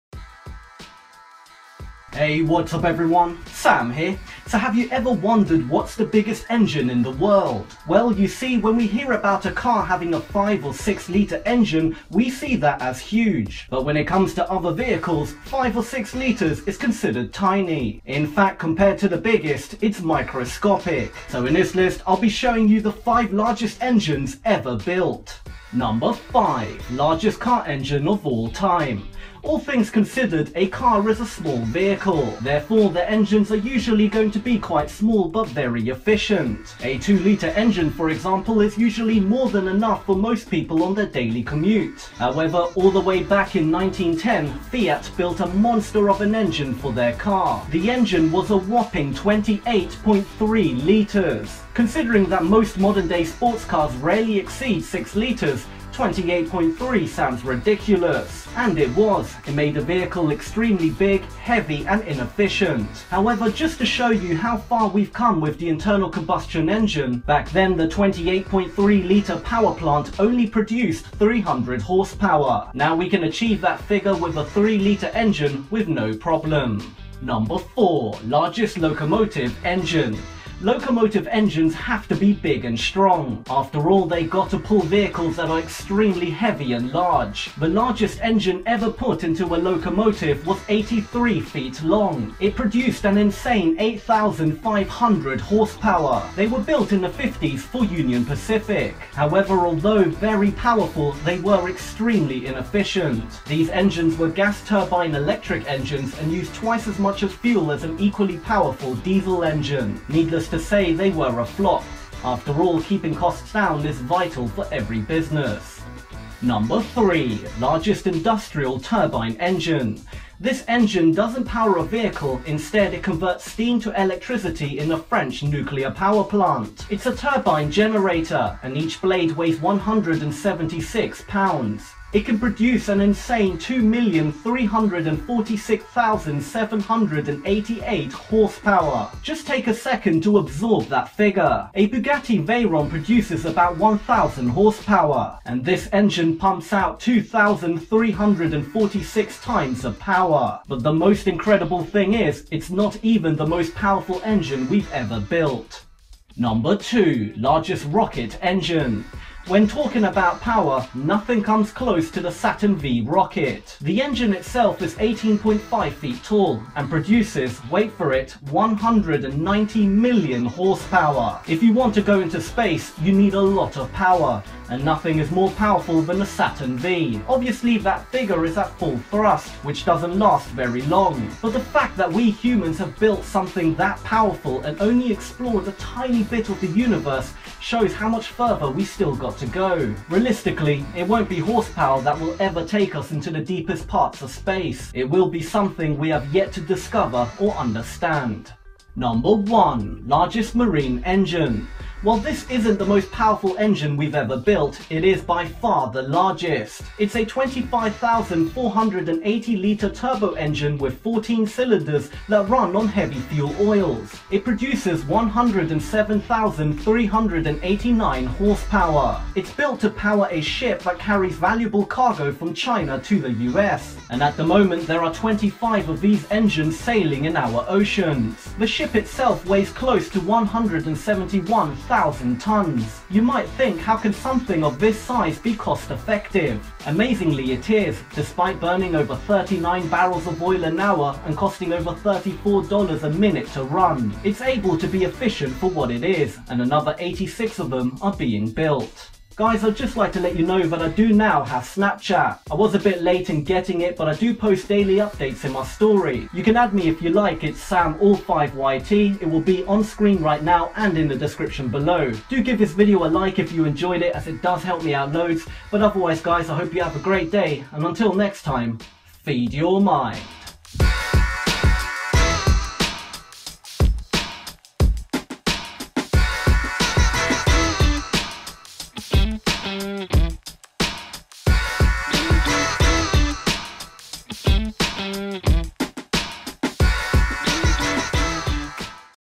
5 Biggest Engines Ever Built!
Have you ever wondered what's the biggest engine in the world?
Well, you see when we hear about a car having a 5 or 6-litre engine we see that as huge.
But when it comes to other vehicles, 5 or 6 litres is considered tiny.
In fact, compared to the biggest, it's microscopic.
So in this list I'll be showing you the 5 largest engines ever built.

5) Largest Car Engine Of All Time

All things considered, a car is a small vehicle.
Therefore their engines are usually going to be quite small but very efficient.
A 2-litre engine, for example, is usually more than enough for most people on their daily commute.
However all the way back in 1910, Fiat built a monster of an engine for their car.
The engine was a whopping 28.3 litres.
Considering most modern day supercars rarely exceed 6 litres, 28.3 sounds ridiculous.
And it was, it made the vehicle extremely big, heavy and inefficient.
However just to show you how far we have come with the internal combustion engine.
Back then the 28.3-litre power plant only produced 300 horsepower.
Now we can achieve that figure in a 3-litre engine with no problem.

4) Largest Locomotive Engine

Locomotive engines have to be big and strong.
After all they got to pull vehicles that are extremely heavy and large.
The largest engine ever put into a locomotive was about 83 feet long.
It produced an insane 8,500 horsepower.
They were built in the 50's for Union Pacific.
However although very powerful they were extremely inefficient.
These engines were gas turbine electric engines and used twice as much fuel as an equally powerful diesel engine.
Needless to say, they were a flop, after all keeping costs down is vital for all businesses.

3) Largest Industrial Turbine Engine

This engine doesn't power a vehicle, instead, it converts steam into electricity at a French nuclear power station.
It's a turbine generator and each blade weigh's 176 pounds.
It can produce an insane 2,346,788 horsepower.
Just take a second to absorb that figure.
A Bugatti Veyron produces only about 1,000 horsepower, this thing pumps out 2,346 times the power.
But the incredible thing is, it's not even the most powerful engine we've ever built.

2) Largest Rocket Engine

When talking about power, nothing comes close to the Saturn V rocket.
The engine itself is 18.5 feet tall, and produces, wait for it 190 million horsepower.
If you want to go to the moon, you need a powerful engine, and nothing is more powerful than the Saturn V.
Obviously, that figure is at full thrust which doesn't last that long.
But the fact we humans have built something that incredibly powerful and only explored a tiny part of the universe shows how much further we got to go.
Realistically it won't be horsepower that will ever take us into the deepest parts of space.
It will be something we have yet to discover or understand.

1) Largest Marine Engine

While this isn't the most powerful engine we ever made, it is by far the largest.
It's a 25,480-litre turbo engine with 14 cylinders that runs on heavy fuel oils.
It produces is 107,389 horsepower.
It's built to power a vessel that carries valuable cargo from China to the U.S.
And at the moment there are 25 of these engines sailing in our oceans.
The ship itself weighs close to 171,000 tonnes.
You might think how can something of this size be cost effective.
Amazingly it is, despite burning 39 barrels of oil an hour and costing over $34 every minute.
It's able to be very efficient for what it is, and another 86 of them are being built.